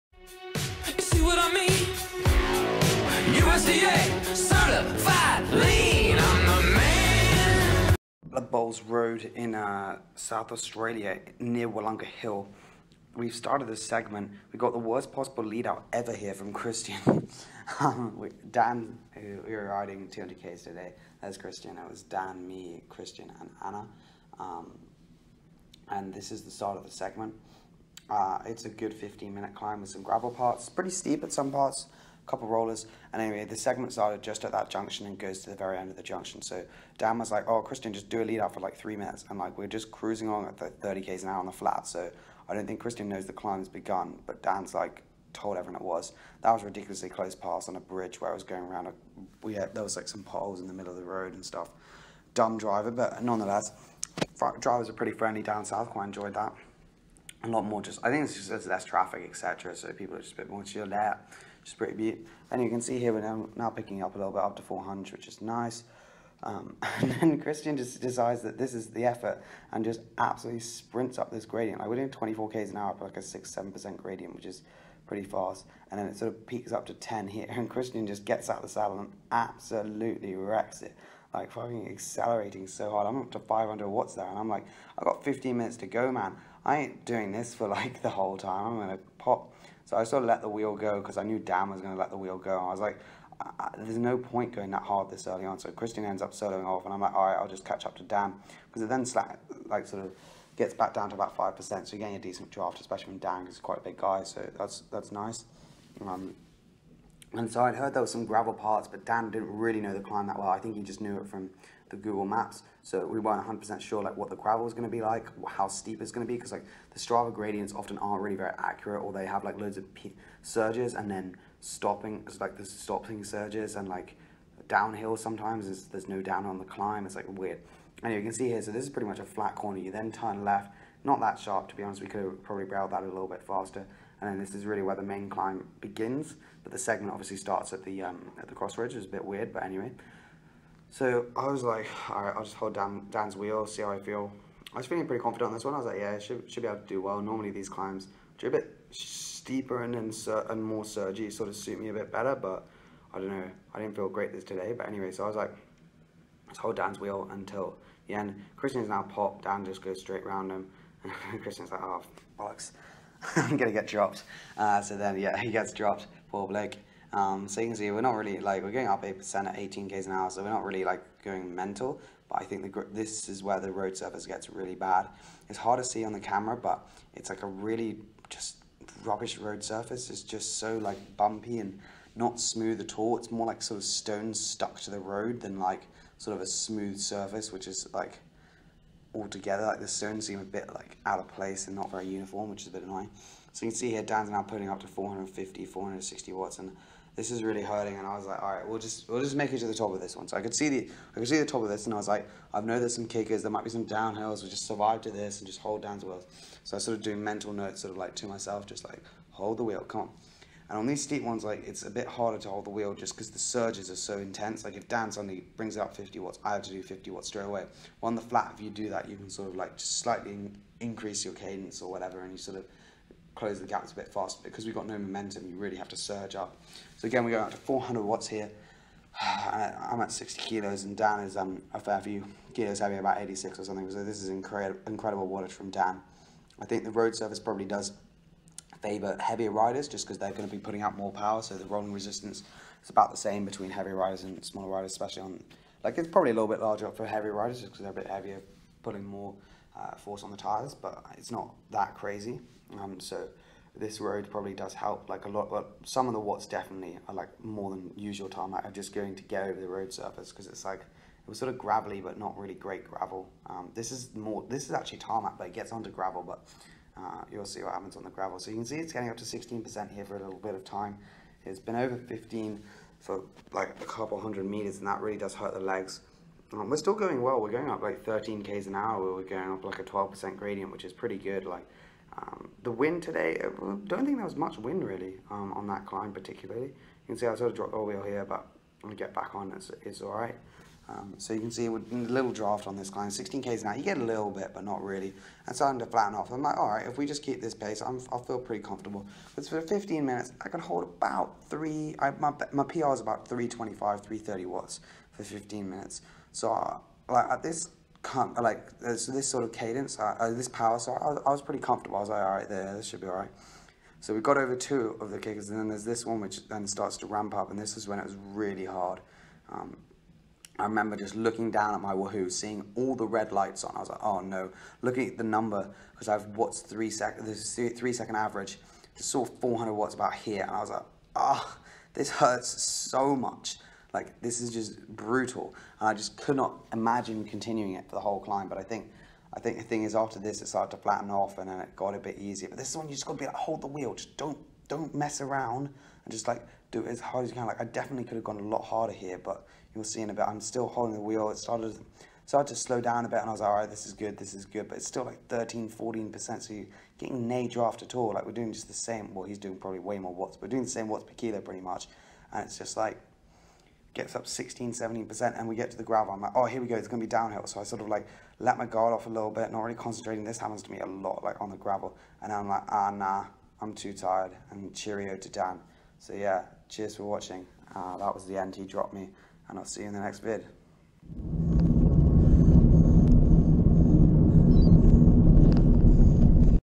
Holding the Wheel up A 15%+ Climb - Tips and Hints
Trying to hold Dan's wheel up a super steep climb , couldn't manage to do it as the surges were too much and I had to be soft and give up. Strava